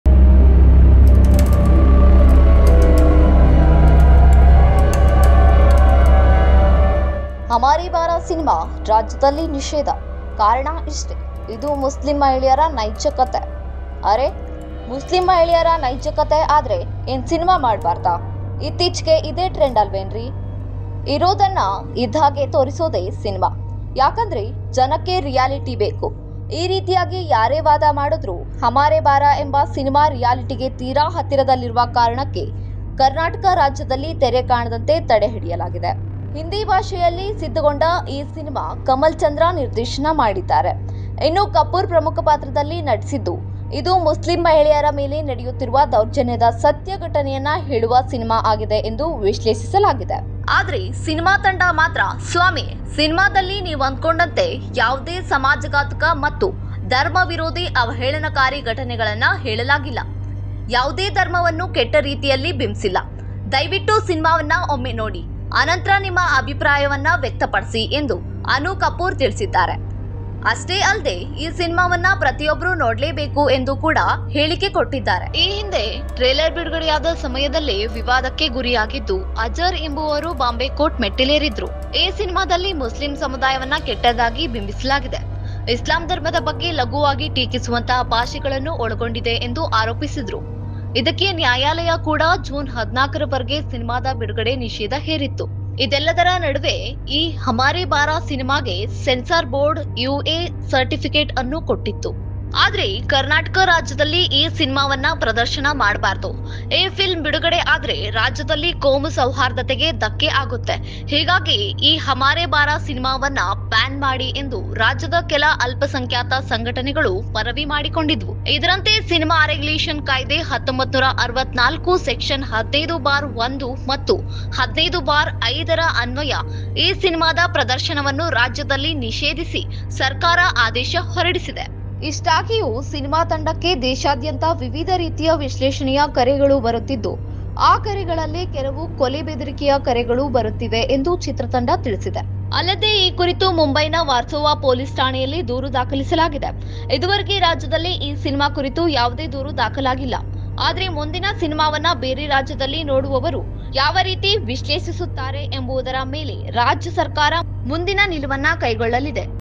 ಹಮಾರೆ ಬಾರ ಸಿನಿಮಾ ಈ ಕಾರಣಕ್ಕೆ ಕರುನಾಡಲ್ಲಿ ನಿಷೇಧ!
ಹಮಾರೆ ಬಾರಾʼ ಸಿನಿಮಾ ರಾಜ್ಯದಲ್ಲಿ ನಿಷೇಧ! ಕಾರಣ ಇದು ಮುಸ್ಲಿಂ ಮಹಿಳೆಯರ ನೈಜ ಕಥೆ?